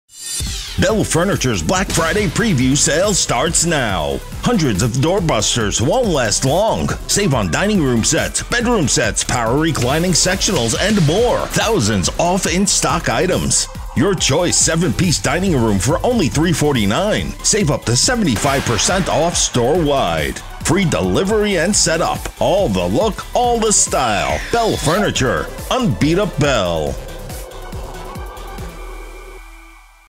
Bel Furniture Black Friday Preview Sale 2024 | Save Up to 75% on Doorbusters!
Get ready for one of the year's biggest savings with Bel Furniture’s Black Friday Preview Sale! Whether shopping for a new living room set, dining room furniture, or bedroom essentials, now is the perfect time to update your home. Don’t miss out—shop online at belfurniture.com or visit one of our showrooms in Katy, Houston, Lake Jackson, San Antonio, Corpus Christi, Sugar Land, Victoria, Clarewood, Deerbrook, and more!